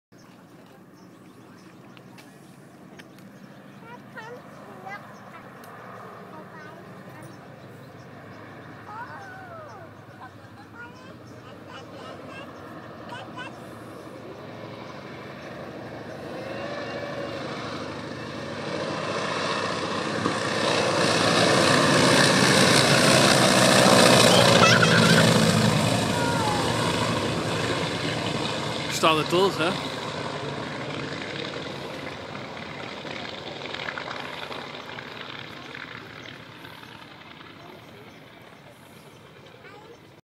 JU 52 startet durch Hahnweide 2.00.2011
Rauch am rechten Motor